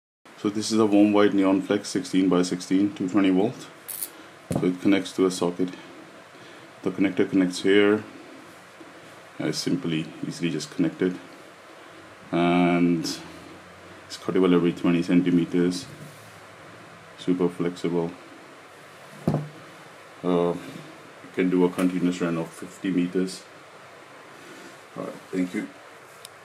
Warm White Neon Flex 16x16mm Top Bend 220V 240V
Warm White 3000K Neon Flex 16x16mm 220V 240V 120 LEDs/m is perfect for creating an infinite amount of environments its silicone cover offers dotless light and performs better than the standard Strip lights with visible individual LED Chips. Because it’s so flexible, its installation is simple and requires clips to installed anywhere and to adapt it to all sorts of shapes in horizontal bending (3cm bending raidus). The Waterproof feature of the Light makes it ideal for Indoor and Outdoor* (Splashproof) use.

Vertical Bend / Top Bend Neon Flex, flexible and cuttable every 20cm.&nbsp;

The 220V 240V Neon Flex is not designed for making neon signs and logos, if you want to make a neon sign or logo please use the&nbsp;LED Neon Flex 12V. &nbsp;The lights are cut from a 50-metre roll and prepared as per your specific requirement.